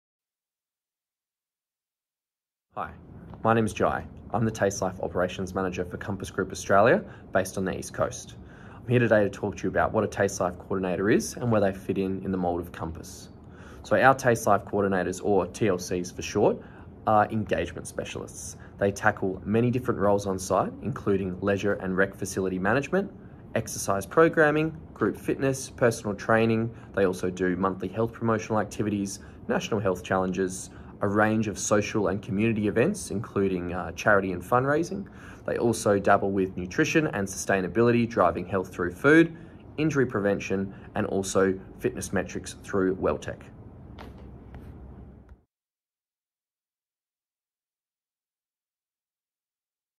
What Is A Taste Life Coordinator
Our Tastelife Operations Manager Jai Twentyman explains the role of a Tastelife Coordinator within a mine site village.

If you'd like to join our teams in Remote Australia, apply now